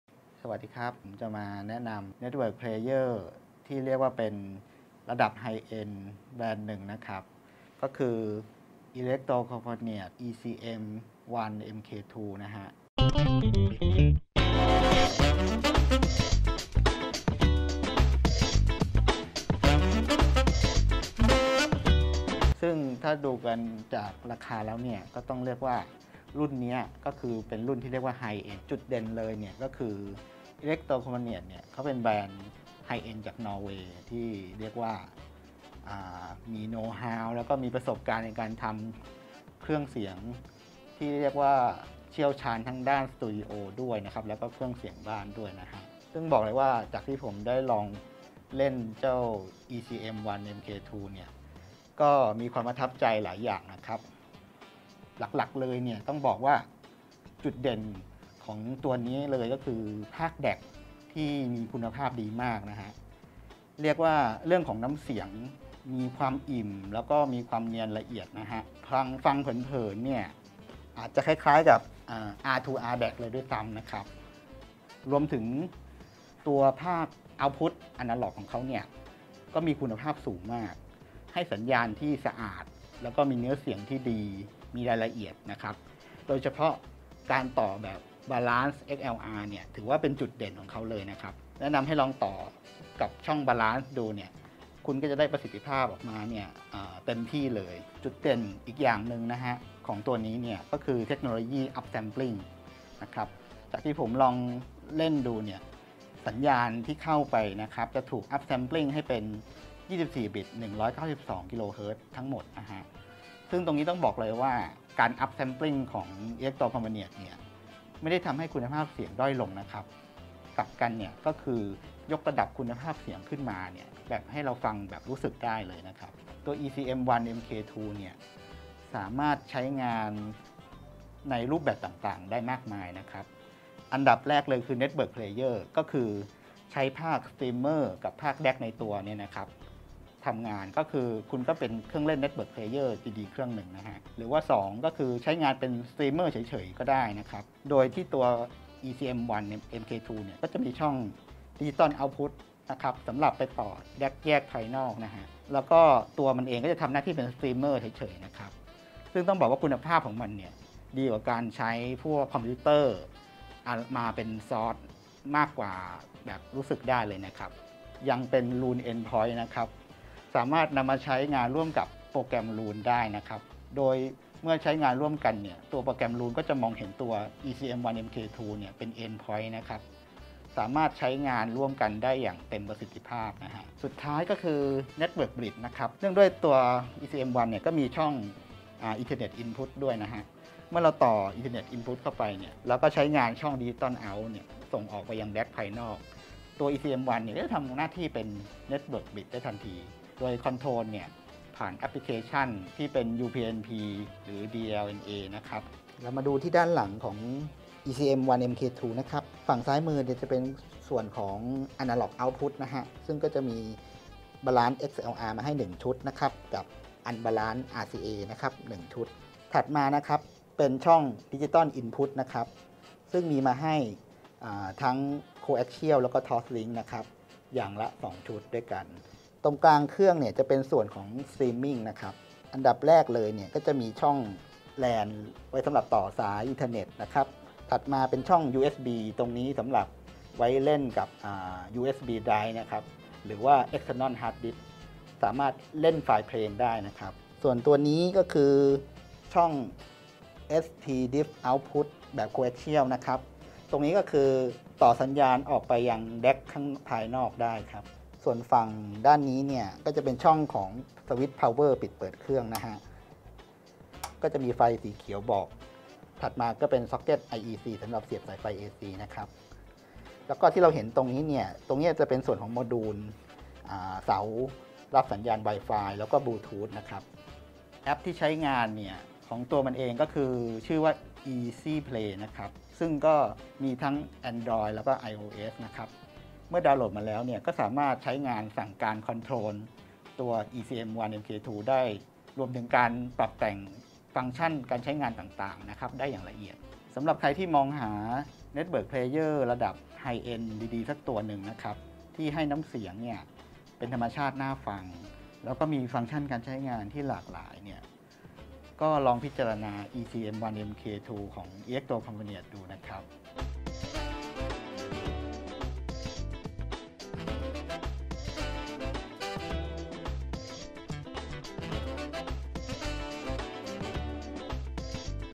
พรีวิว Electrocompaniet EMC1 MKII
พรีวิว ECM1 MKII ไฮเอ็นด์แด็คและมิวสิคสตรีมเมอร์ น้ำเสียงเนียน ทรงพลัง จาก Electrocompaniet ผู้ผลิตที่เข้าถึงแก่นของความเป็นดนตรี